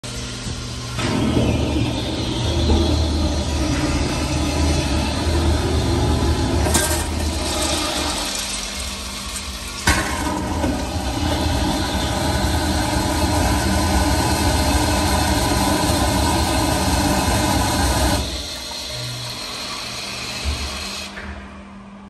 AcornVac toilet
Filmed at Moynihan Train Hall/Moynihan Food Hall at New York Penn Station in New York City